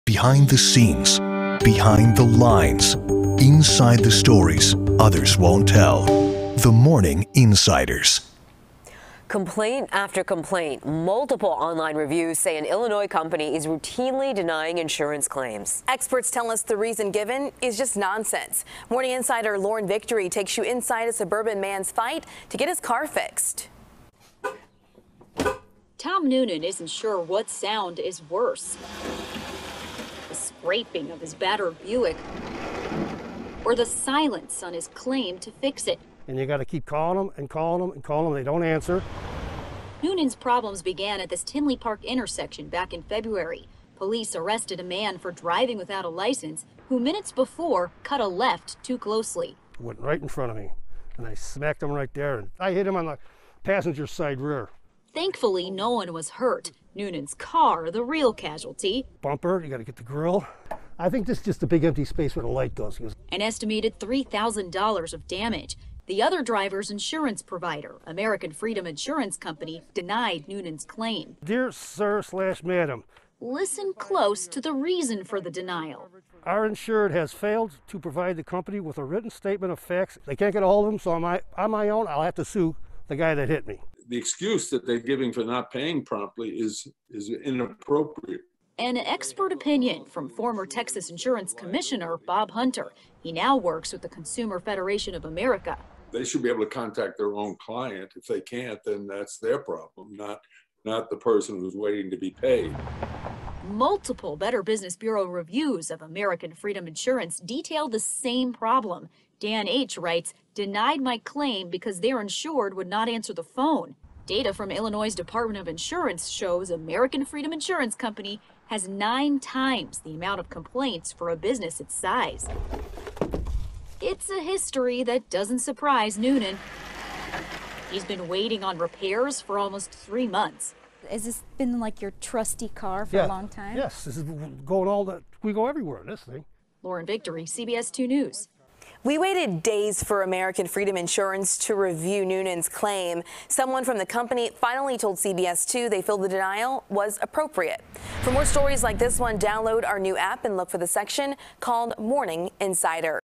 American Freedom Insurance Company Refusing To Pay For Crash Victim's Repairs After They Can't Reach
His problems began at a Tinley Park intersection back in February. Police arrested a man for driving without a license, who – minutes before – cut a left too closely.